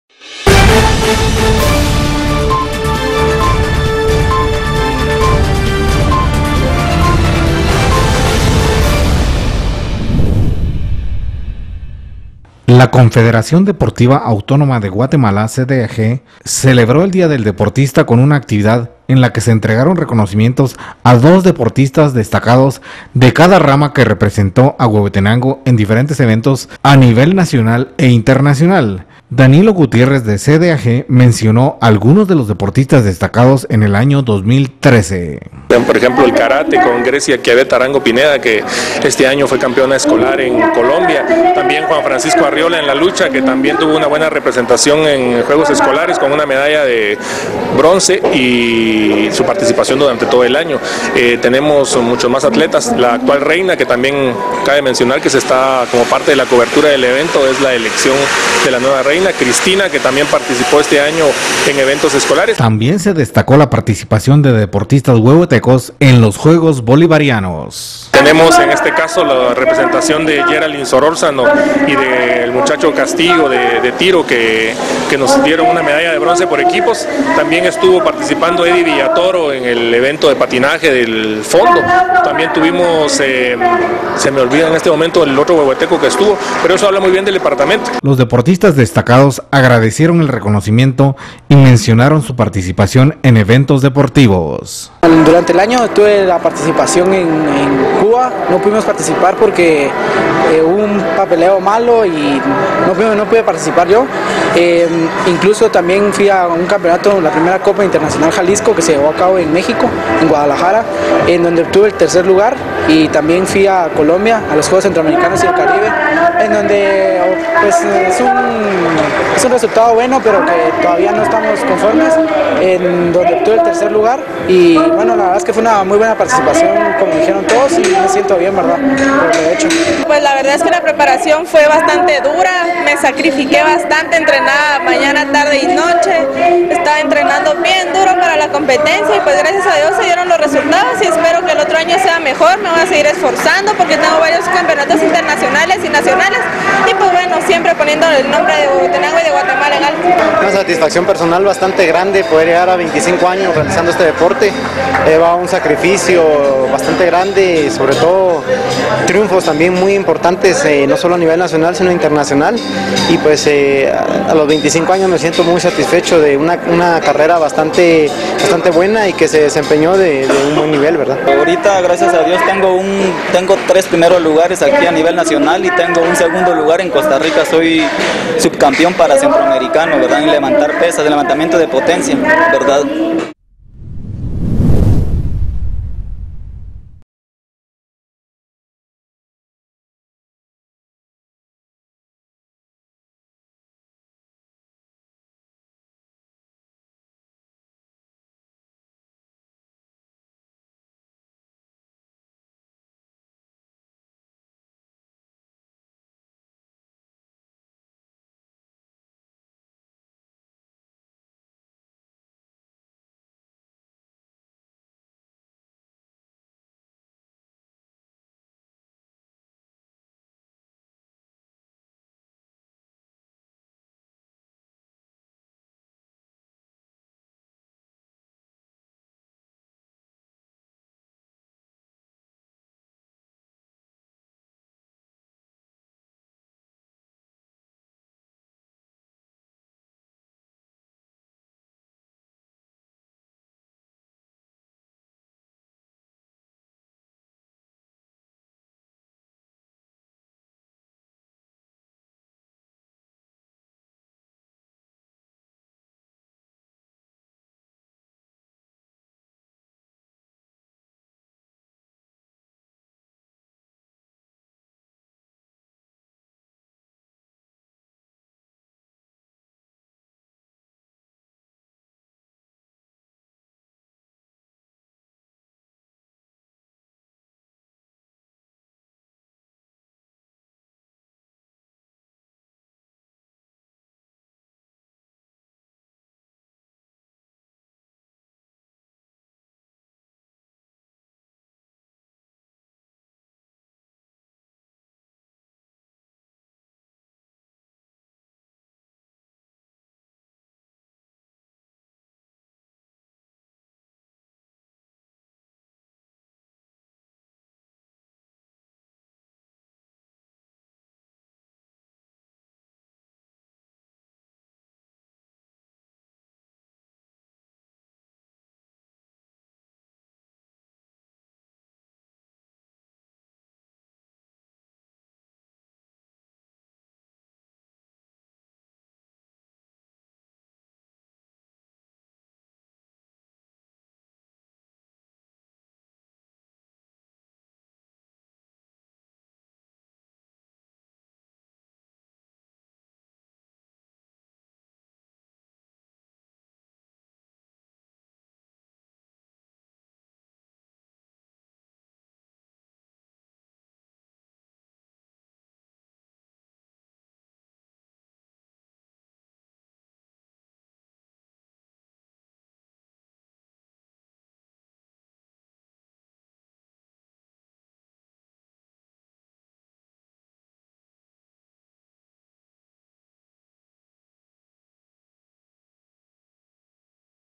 Entregan reconocimiento a deportistas destacados de huehuetenango
La CDAG entrego un reconocimiento a deportistas huehuetecos que destacaron en eventos a nivel nacional e internacional en el presente año.